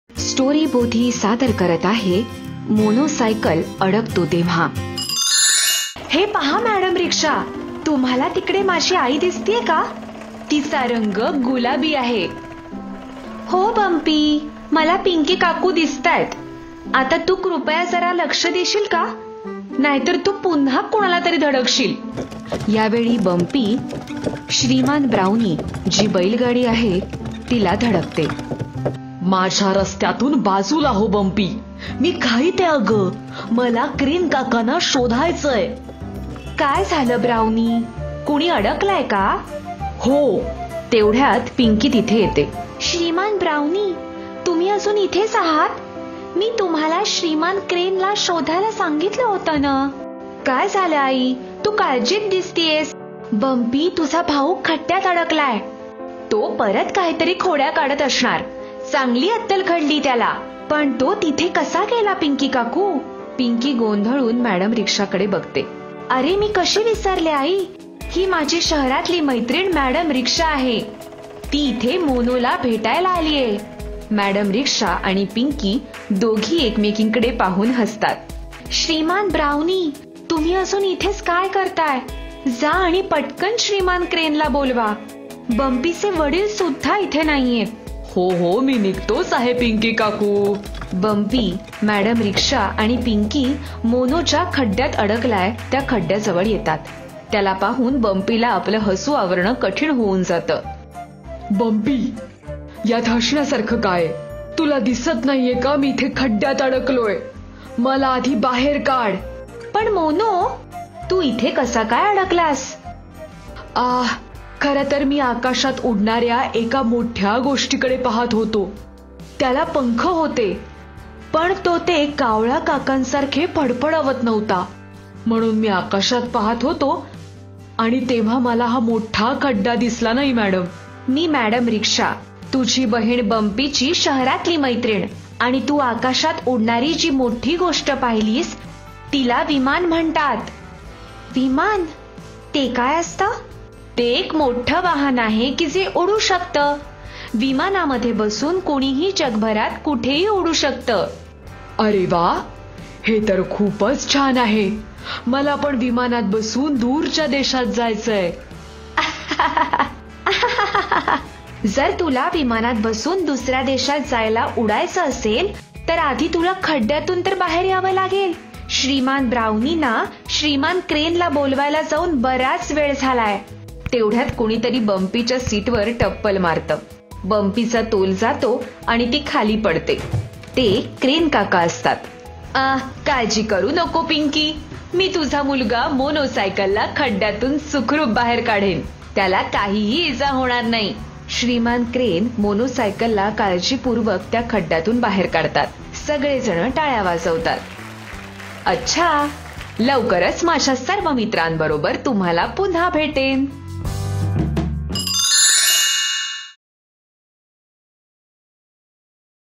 मोनो सायकल अडकतो तेव्हा
Here comes the Monocycle the brother of Bumpy Bicycle , Madam rickshaw and Bumpy Bicycle had gone to garage to meet Monocycle but do you know what they saw!!